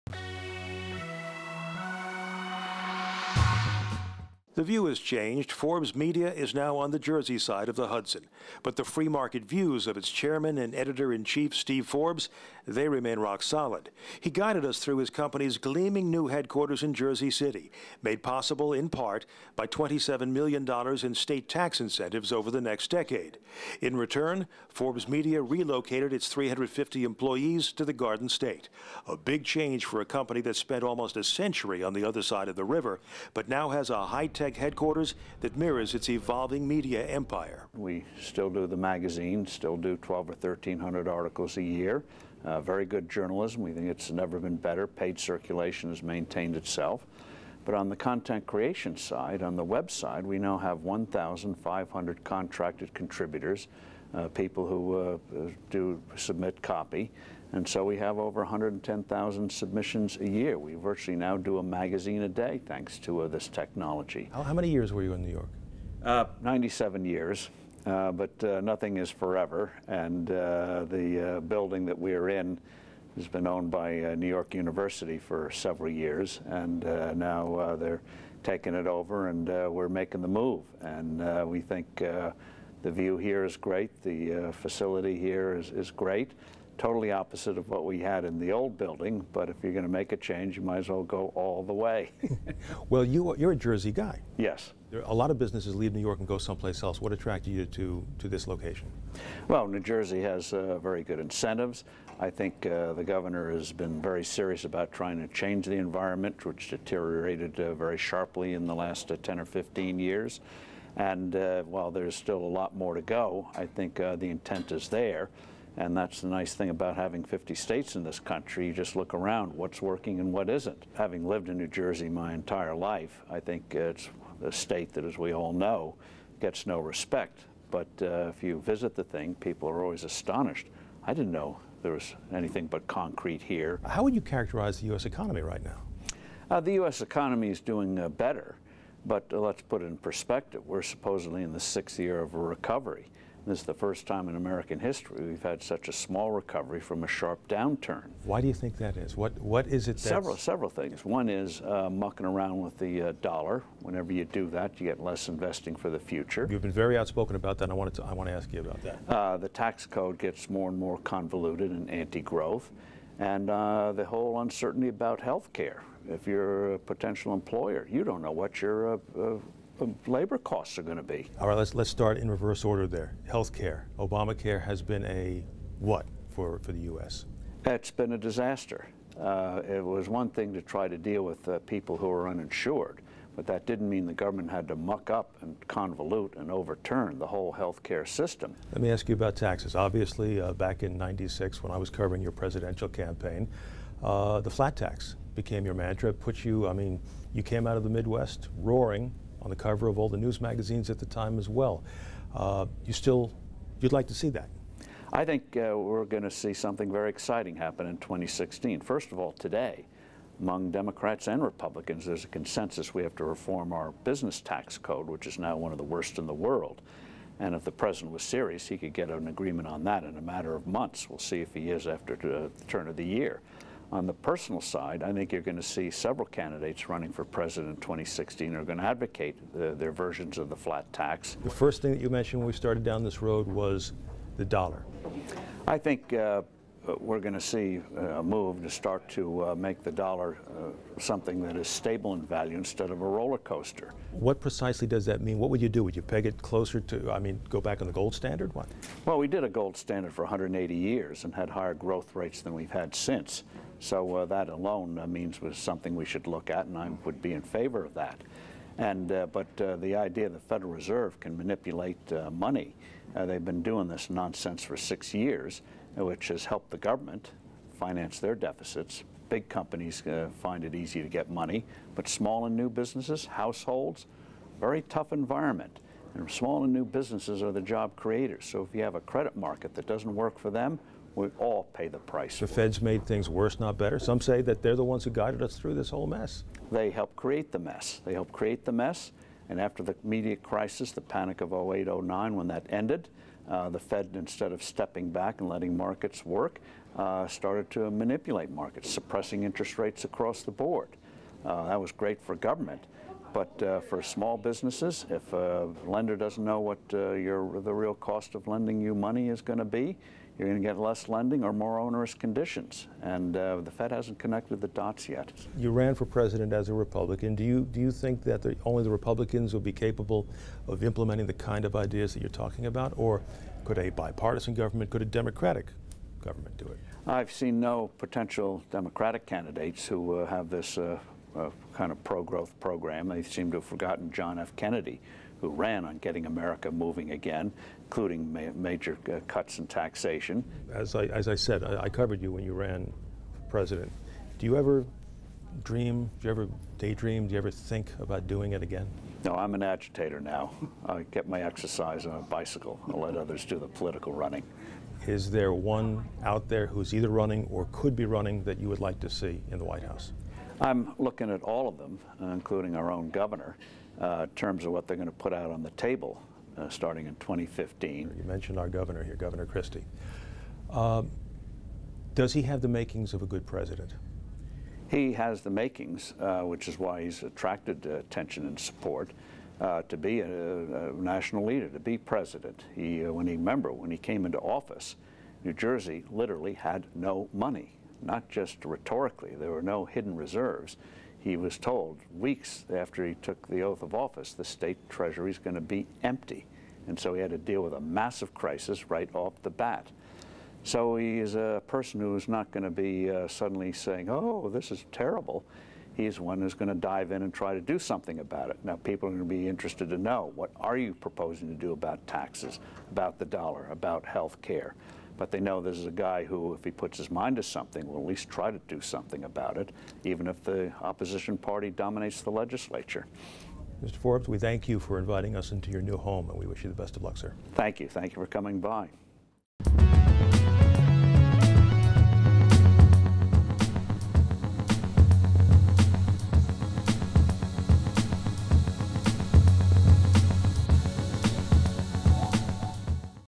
Forbes Moves Across the Hudson to Jersey City
The Forbes Media headquarters has moved to Jersey City, after operating in New York for 97 years. Chairman and Editor-in-Chief Steve Forbes sat down with NJTV News Senior Correspondent Mike Schneider to discuss the move and the climate in New Jersey and the nation. For more New Jersey news, visit NJTV News online at njtvnews.org.